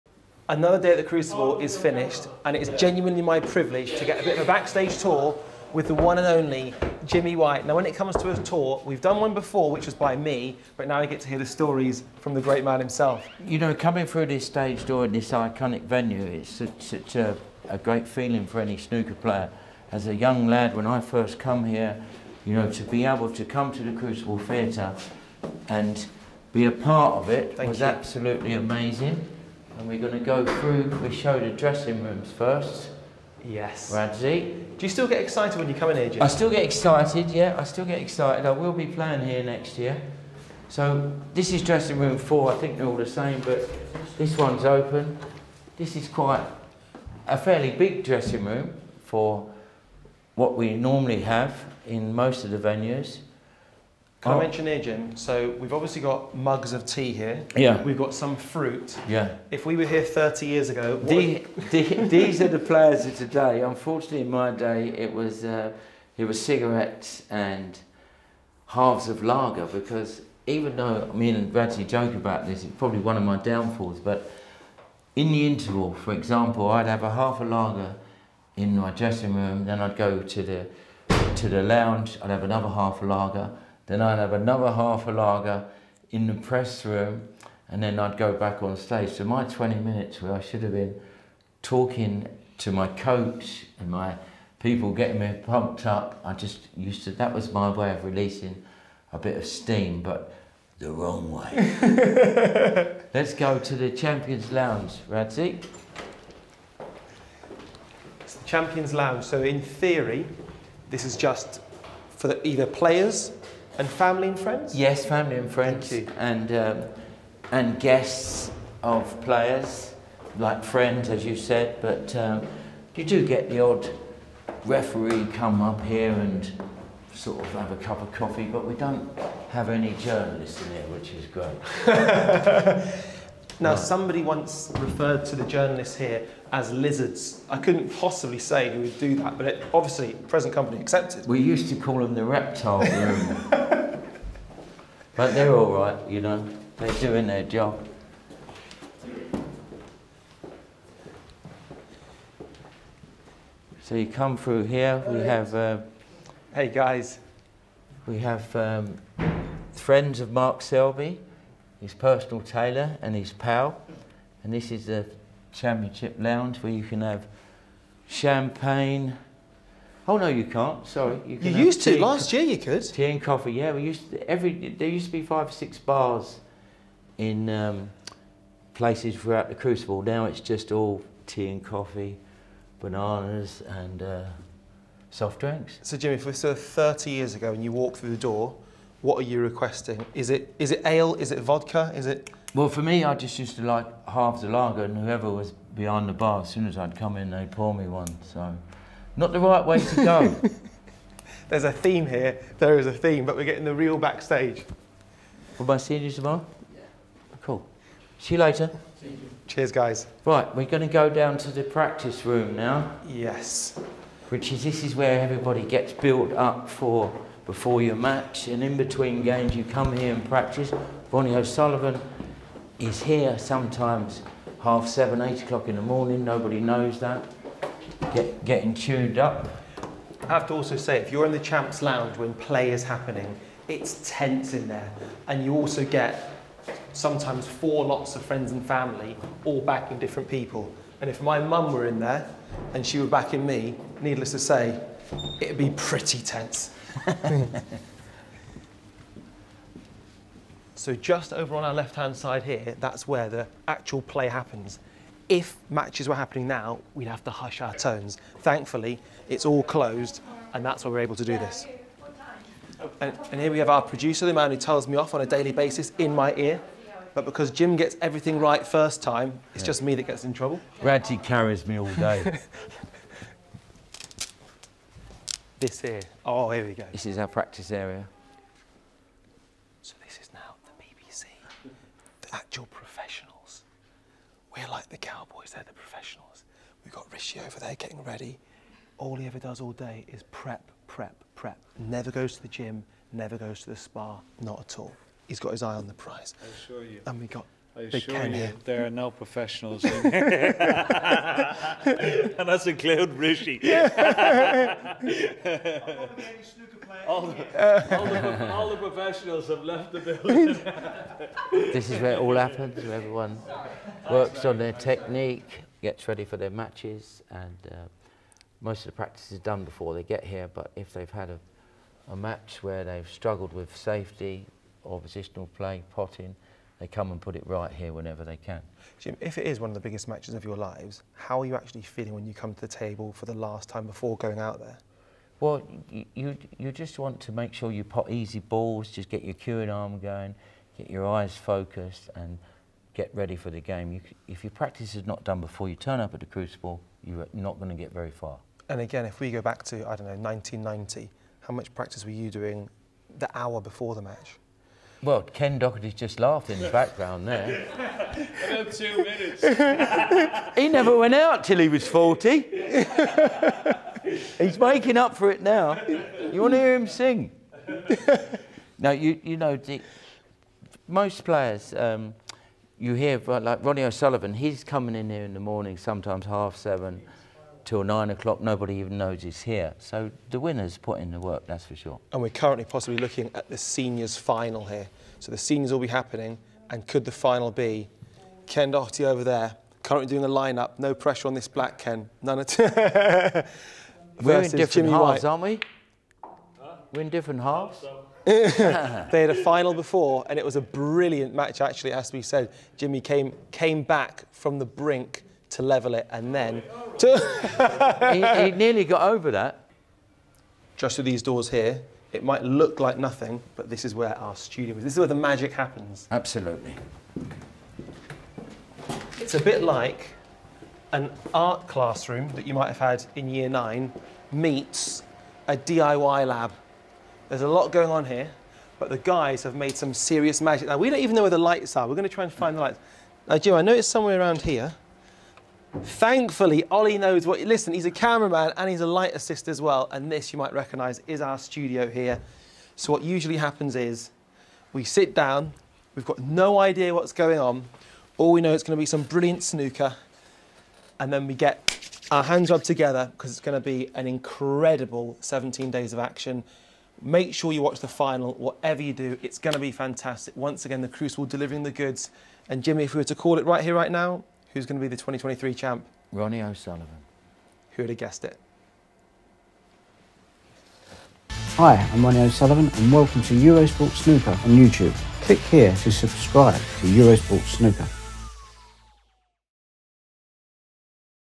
Behind The Scenes! After Hours Backstage Tour Of The Crucible With White & Radzi | Eurosport Snooker
Check out this fascinating video as Jimmy White and Radzi Chinyanganya take us behind the scenes at the historic Crucible.